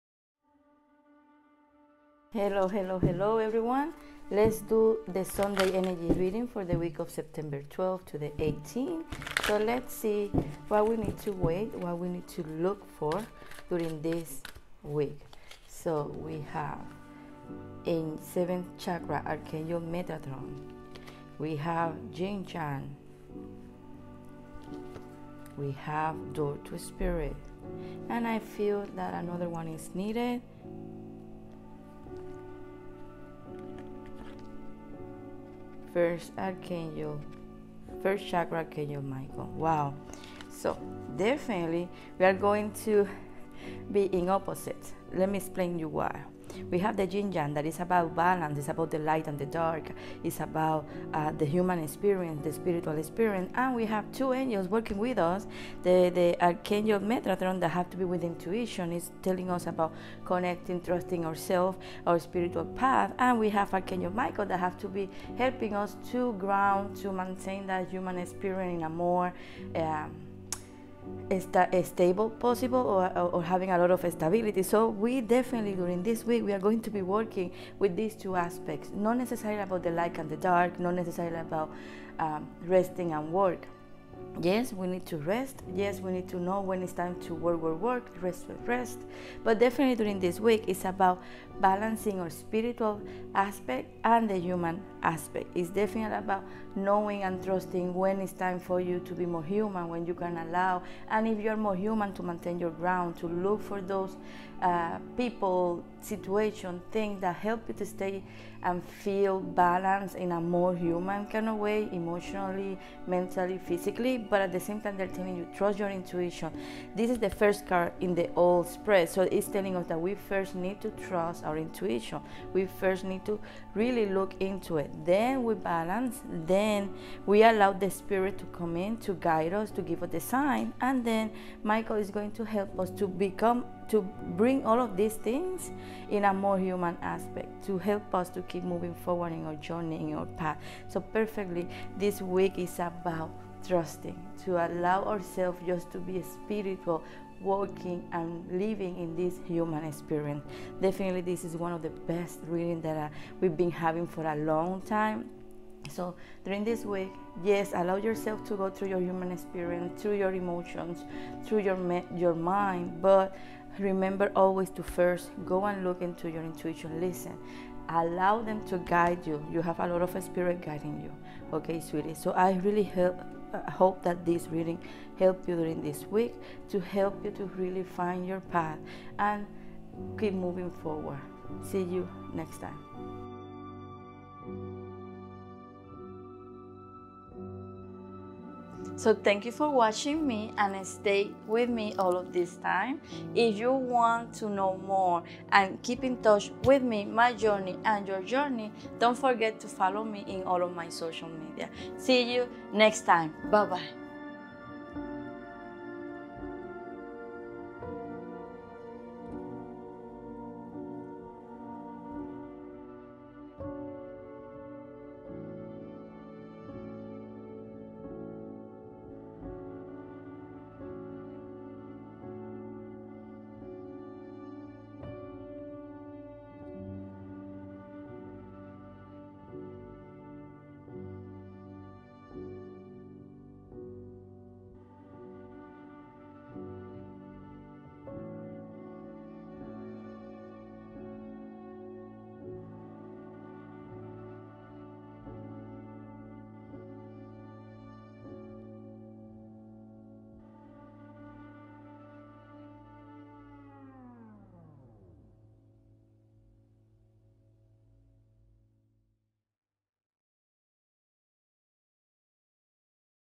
Sunday Energy Reading for the week of Sept 12-18/2021
During this week we are encourage to balance ourselves in all of our aspects. We need to work with our intuition and grounding aspects specifically. During this week we need to observe ourselves and notice what aspects need work.